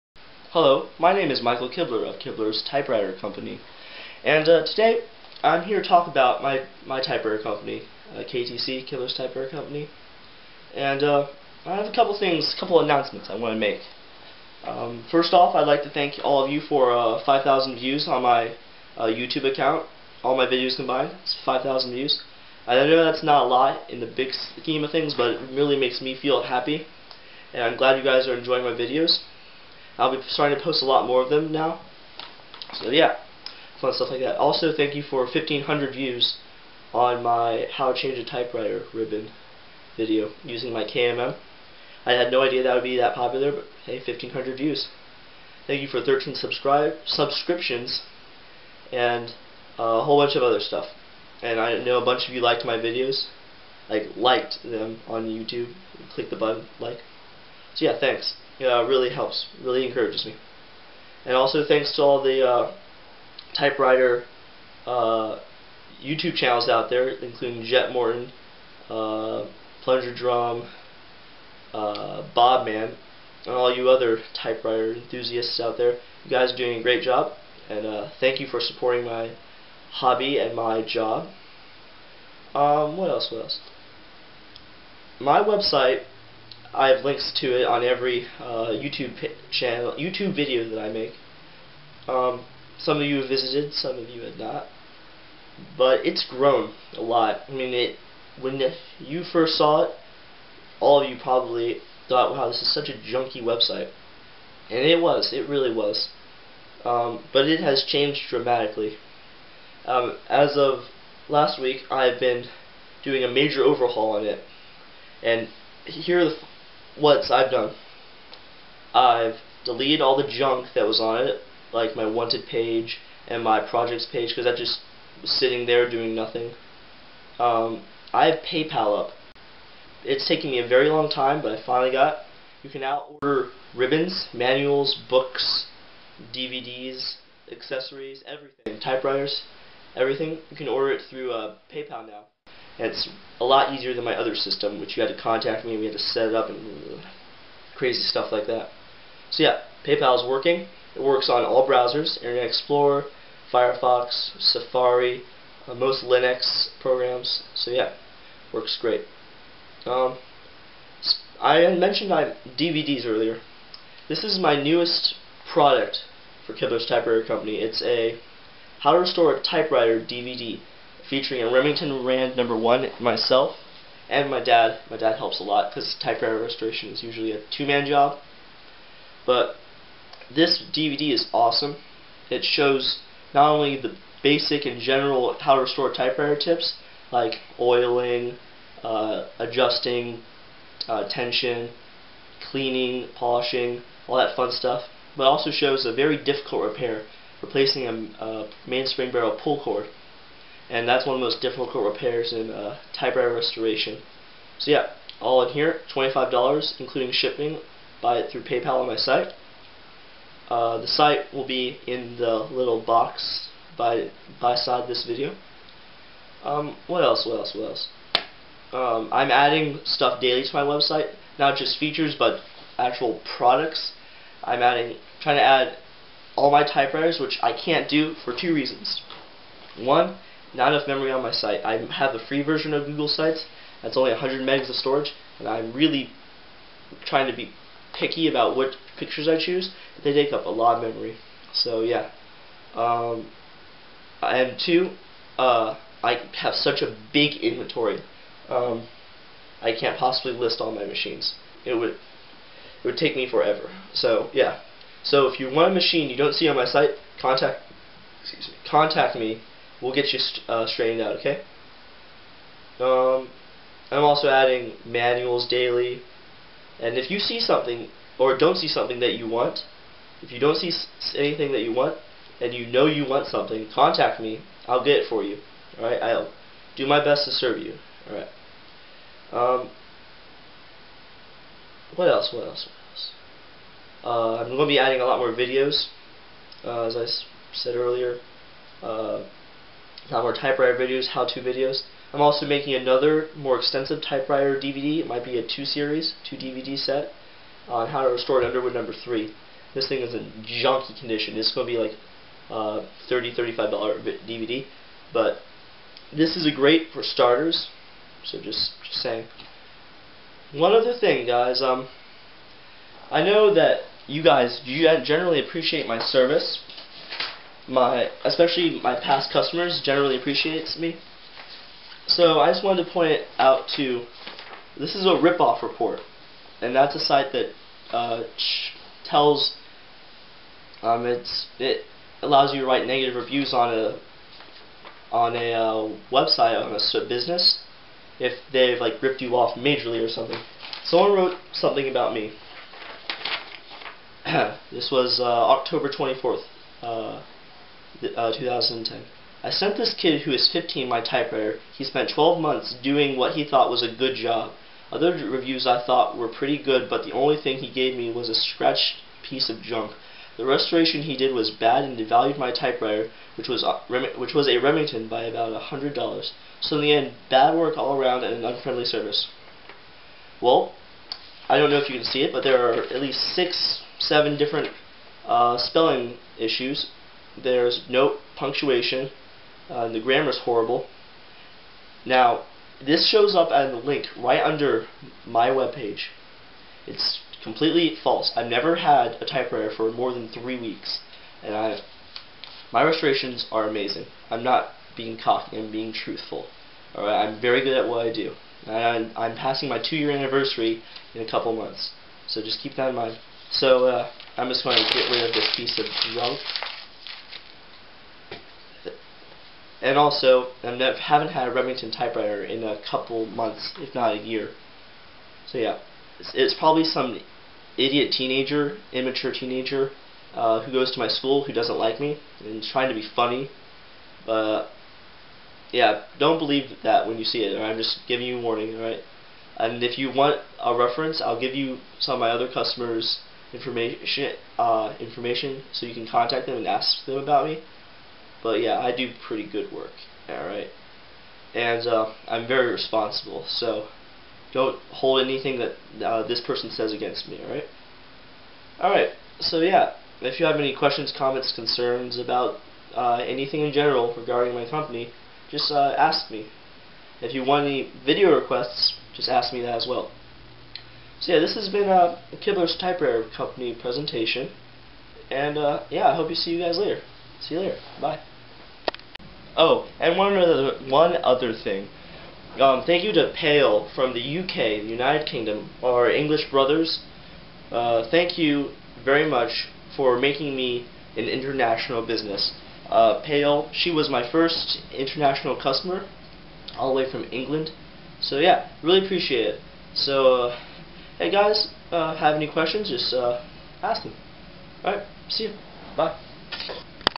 Kibler's Typewriter Co. Update November 2010
Just an update on my company; I just overhauled my website and am adding much more inventory.  visit here at:

or google "kibler's typewriter co."
If you have a video request, ask me and I will see what I can do.